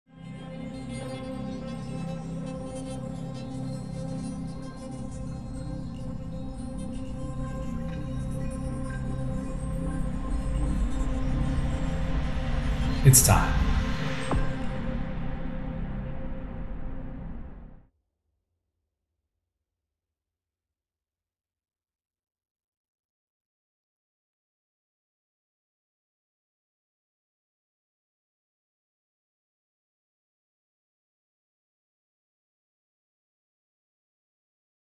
Blackland Prairie Gold Texas Straight Bourbon - It's Time (Bottling)
Aged for 3 years, Blackland Distillery in Fort Worth crafts their latest hand-crafted spirit— a real Texas Straight Bourbon.